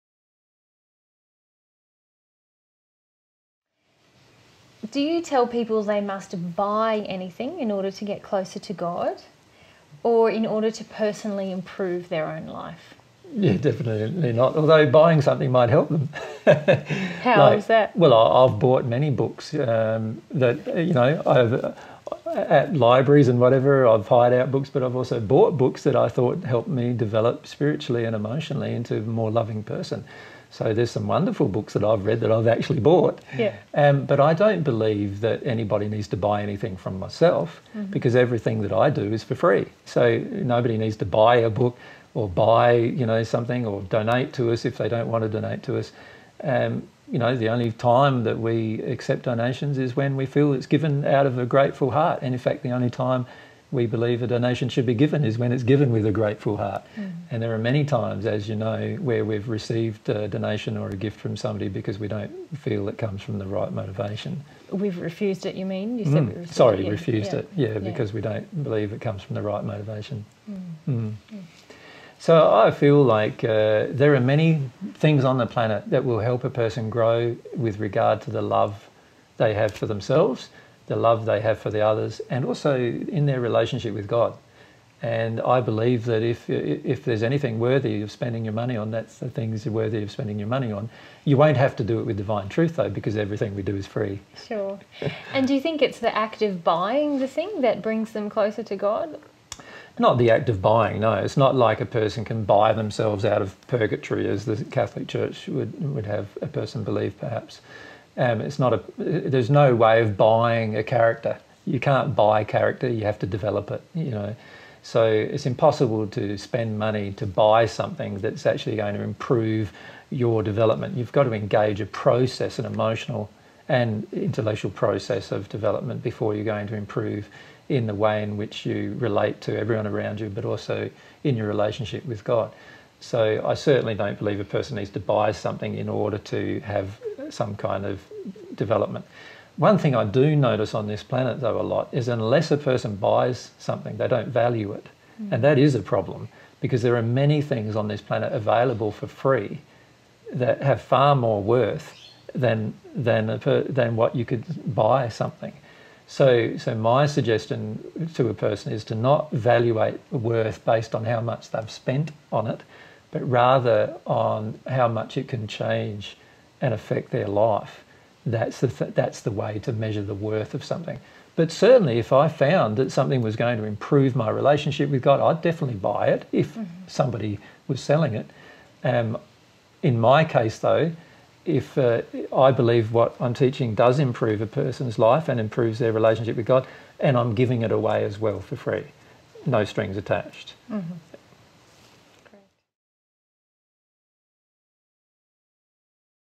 Must people buy things from you to get closer to God?
06. Do you tell people they must buy anything in order to get closer to God, or in order to personally improve their own life?

20130106 Religion General S01Q06

Recorded on 06th January 2013 in Wilkesdale QLD, Australia